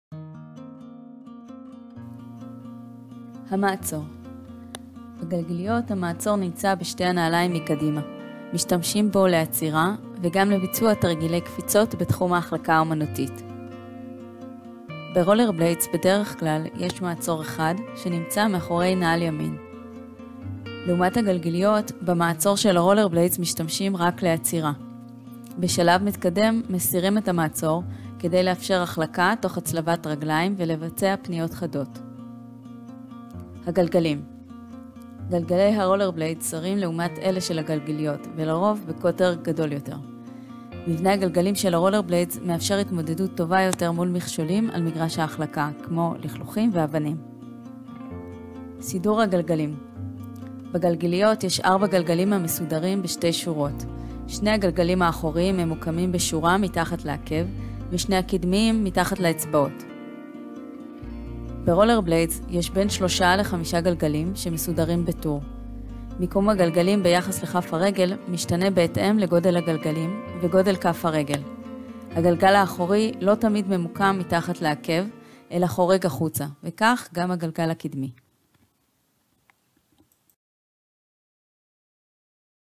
רולר בליידס וגלגיליות - מה ההבדלים ביניהם?
יש הבדלים בין רולר בליידס לגלגיליות (סקטים) במעצור, במבנה הגלגלים, בגודל ובסידור שלהם ביחס לנעל. עוד על רולר בליידס וגלגיליות